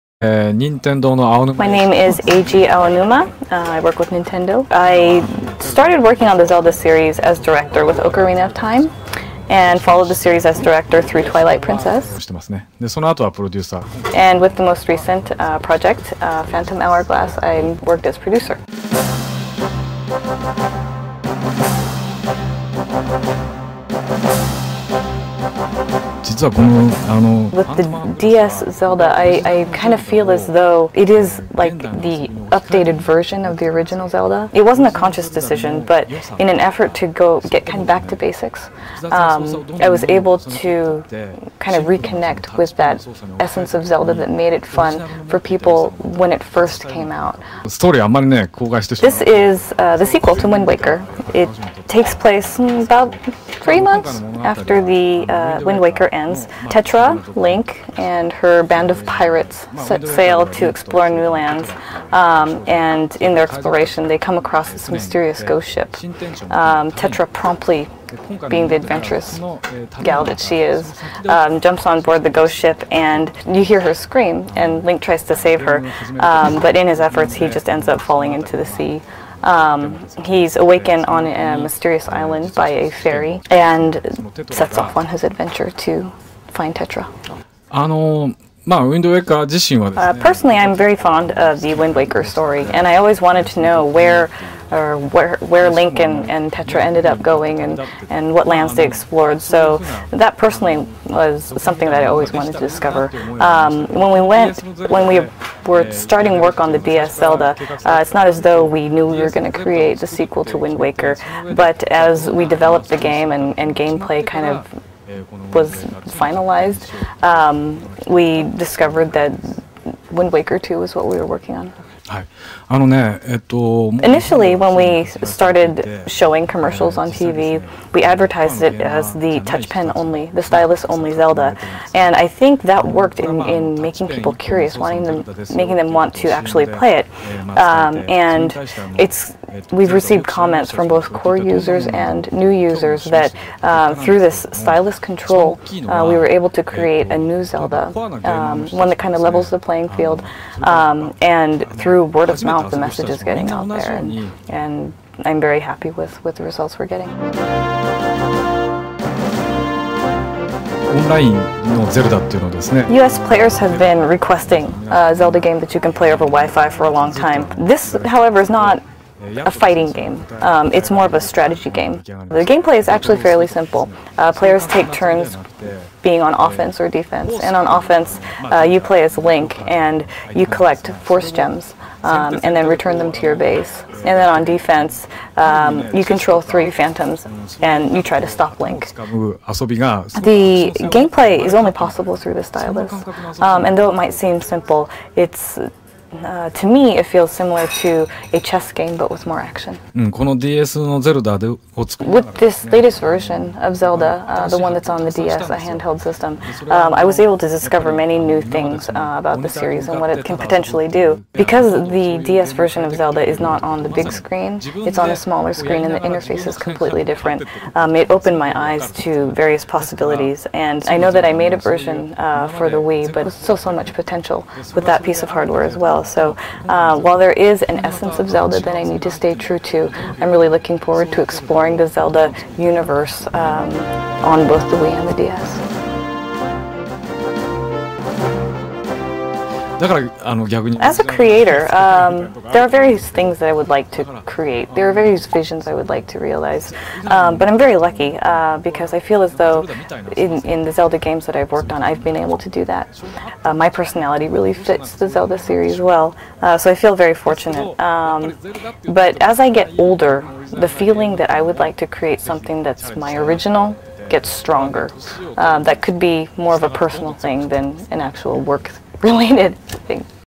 Phantom Hourglass - Interview with Eiji Aonuma
A recent interview with "The Legend of Zelda: Phantom Hourglass"'s producer, Eiji Aonuma.